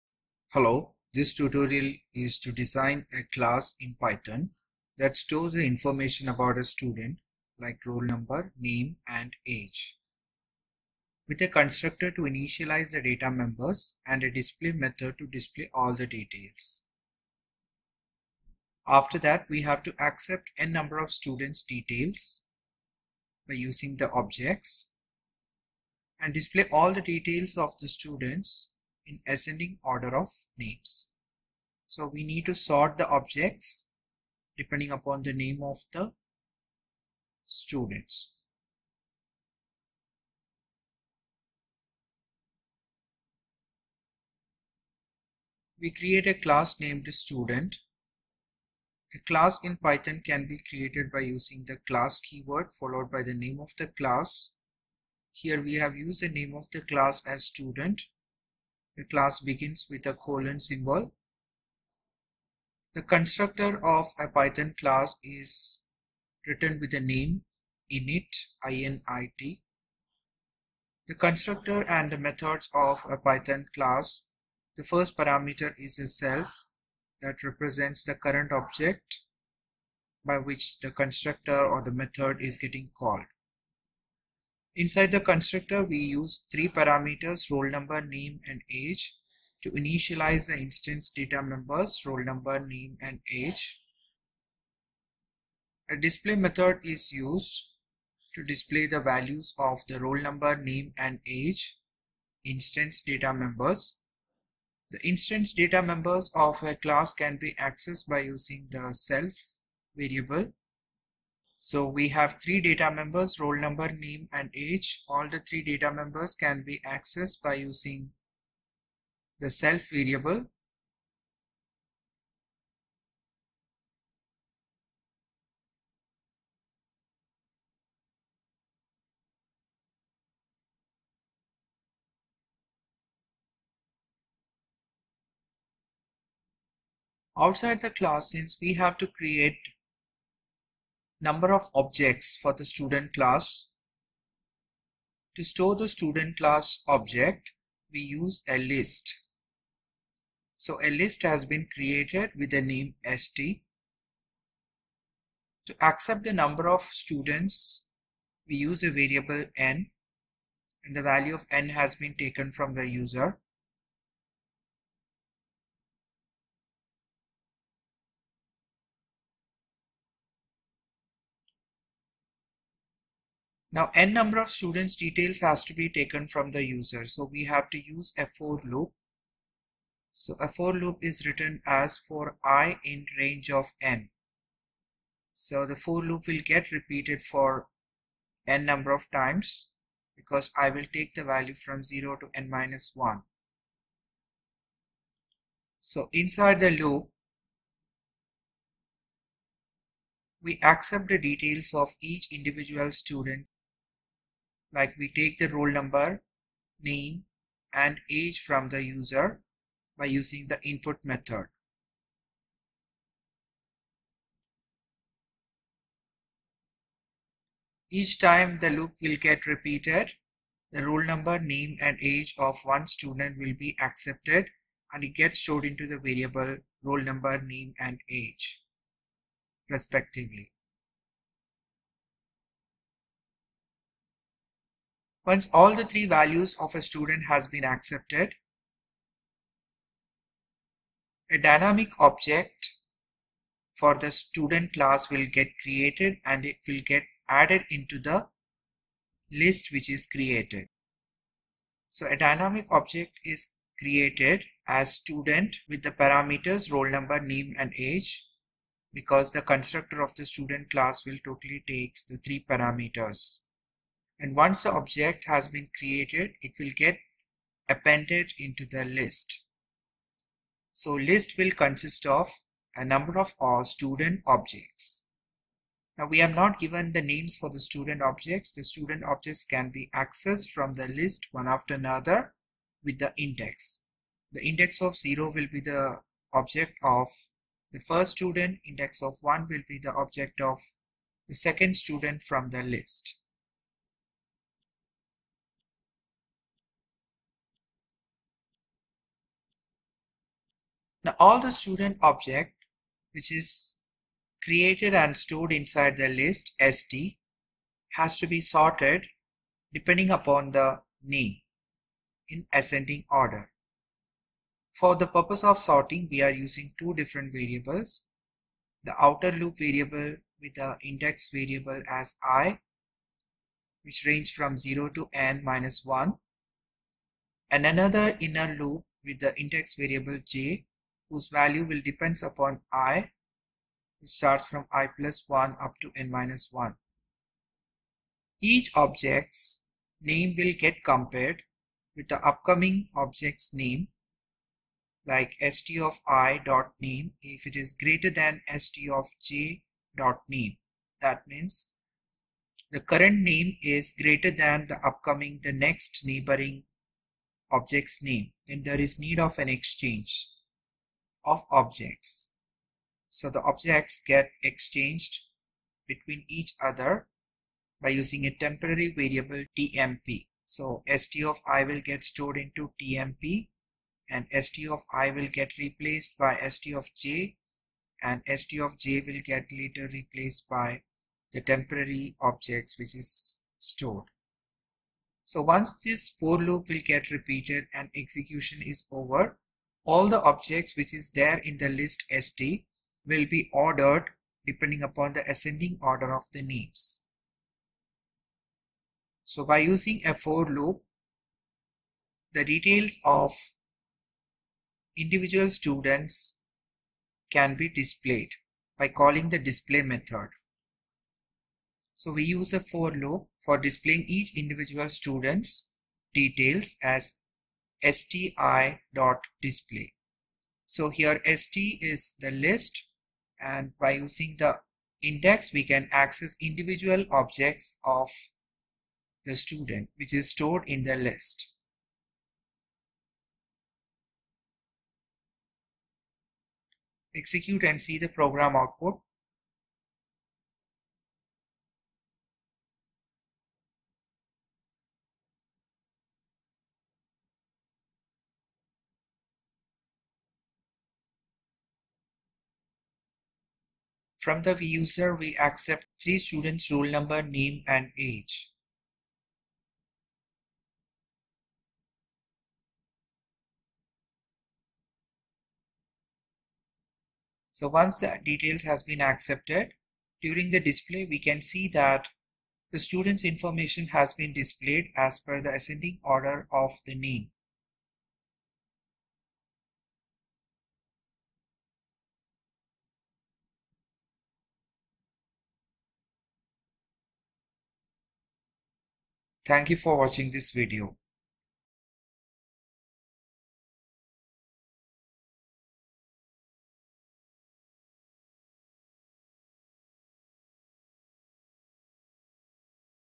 Python Program - multiple objects
Design a class that store the information of Student (rollno, name and age) with a constructor to initialize the data member’s and display() method to display all the details.
1) Accept n-number of students details
2) Display the details of all the students in ascending order of  name